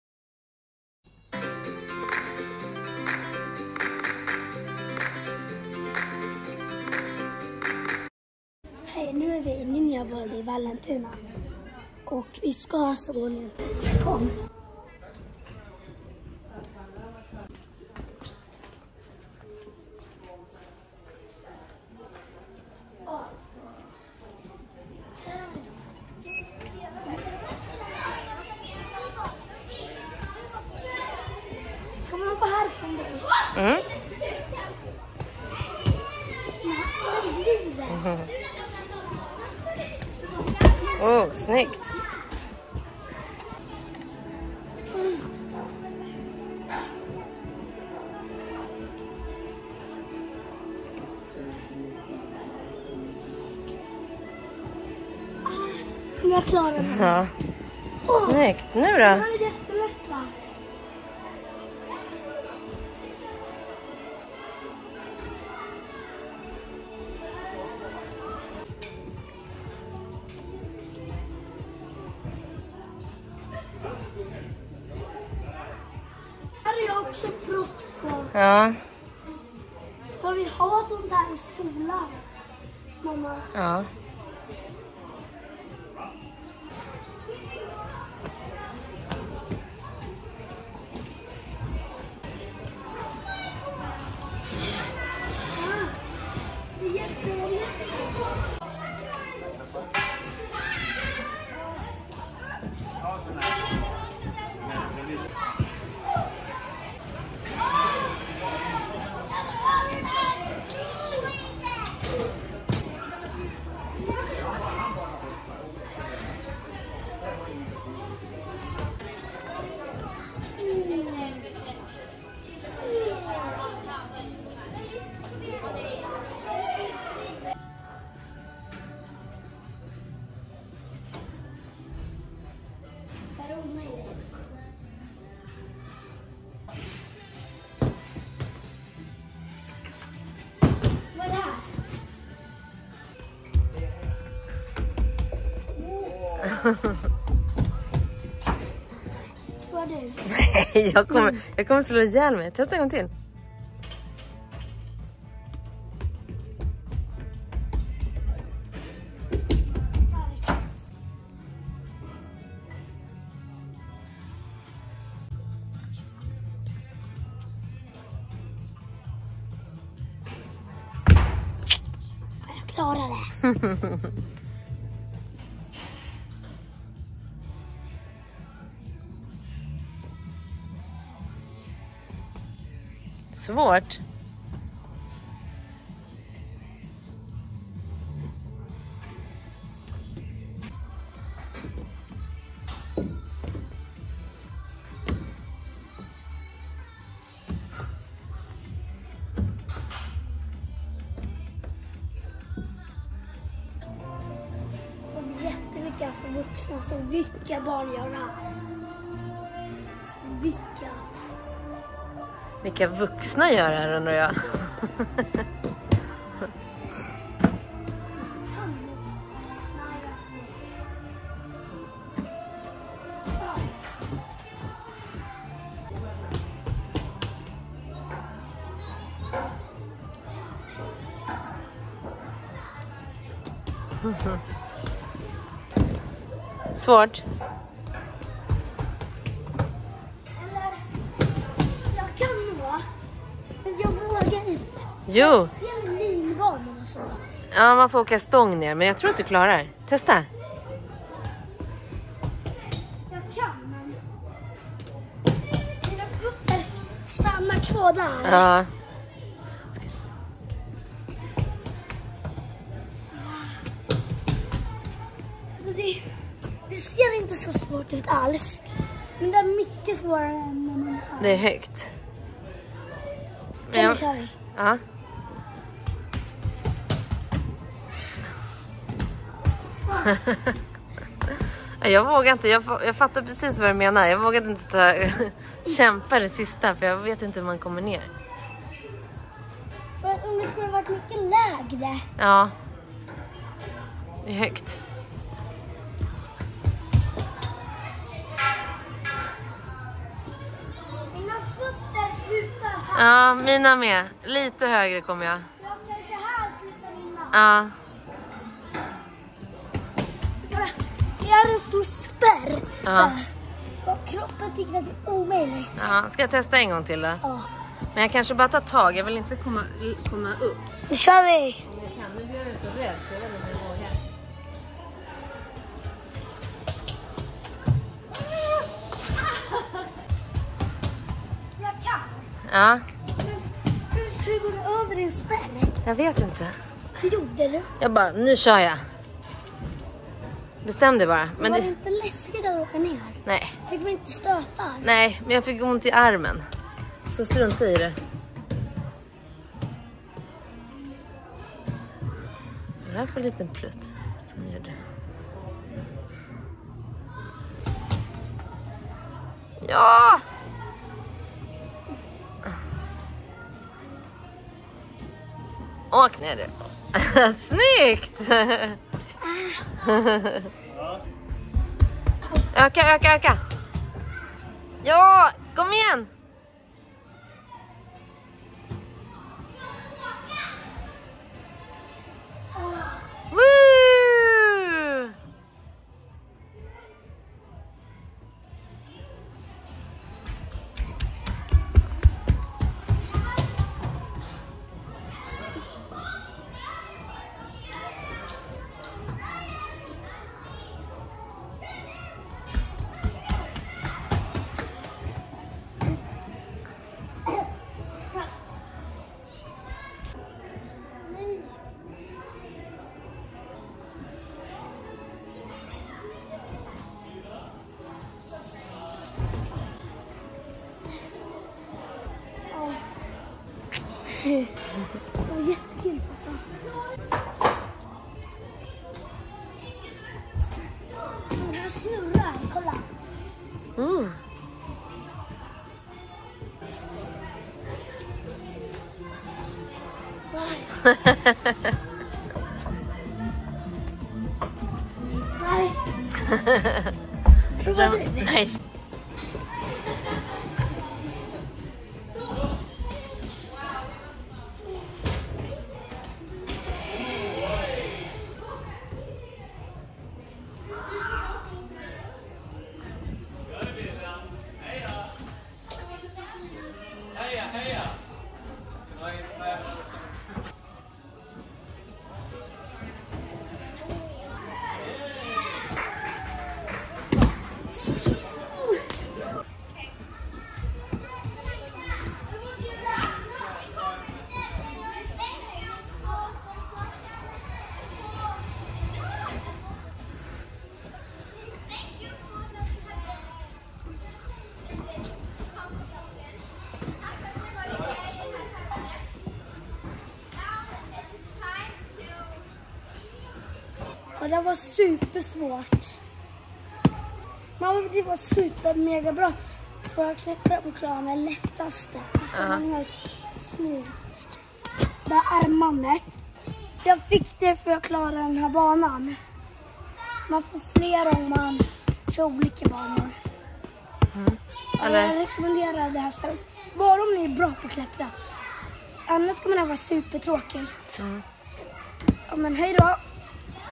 Zack besöker Ninja Park
Zack är på Ninja Park i Vallentuna som är Europas största inomhushinderbana. Det finns 130 hinder för både barn och vuxna, parkourbana, hinderbanor av olika svårighetsgrad, OCR, klättervägg och till och med gym och simhall (fast det testade inte Zack). Det var ganska svåra hinderbanor så för att det ska vara kul krävs det att man är rätt bra på att klättra!